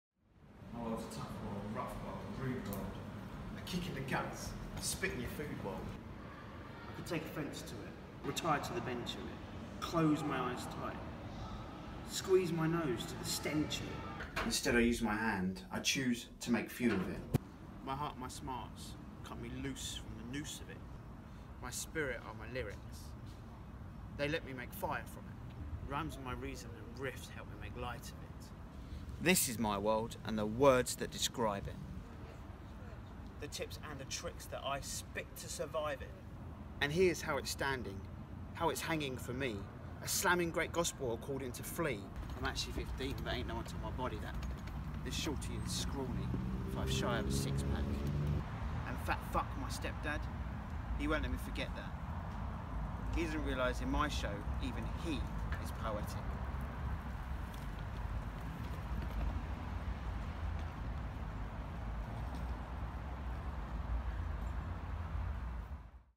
Class Act Audition Tape: Gareth Cooper reading Flea a BBC iPlayer Short
This is my audition tape to join the BBC Class act scheme.